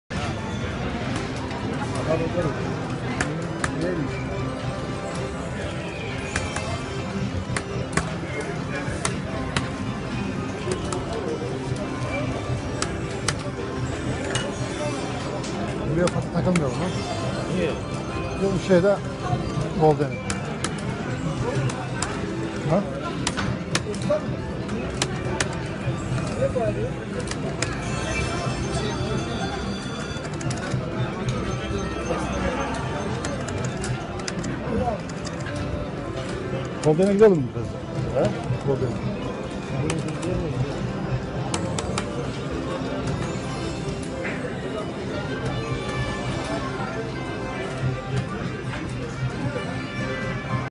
Ak parti Muğla milletvekili kumar oynarken görüntülendi
AKP 26.Dönem Muğla Milletvekili Nihat Öztürk'ün kumarhanede çekilen görüntüleri basına sızdı